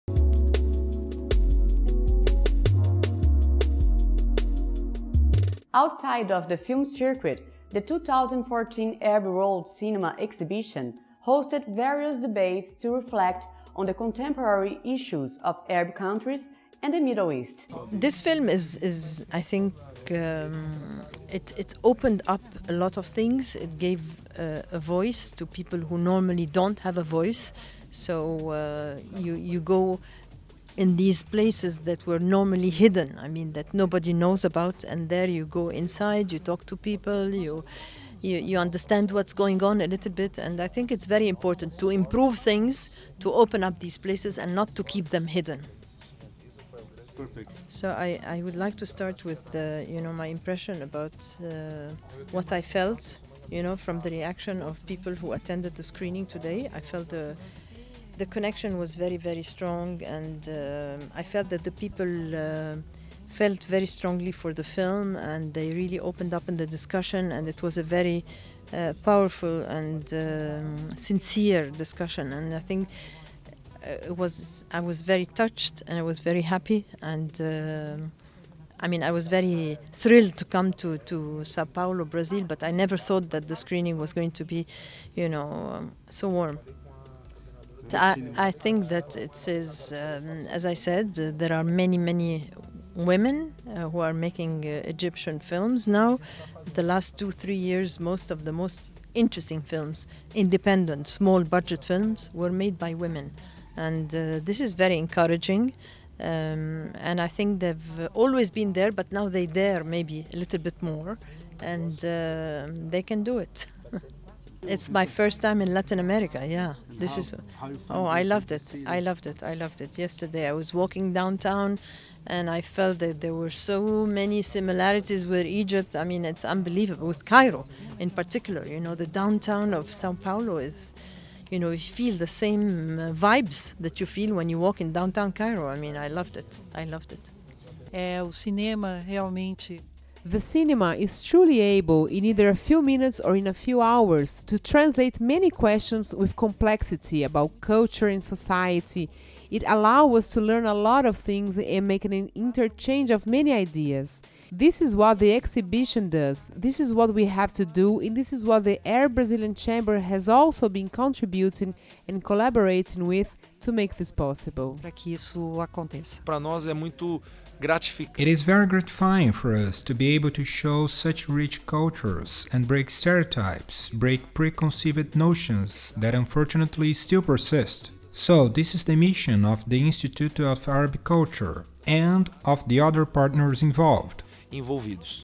Debate on Egyptian Cinema | Culture
Outside of the film circuit, the 2014 Arab World Cinema Exhibition hosted various debates to reflect on the contemporary issues of Arab countries and the Middle East.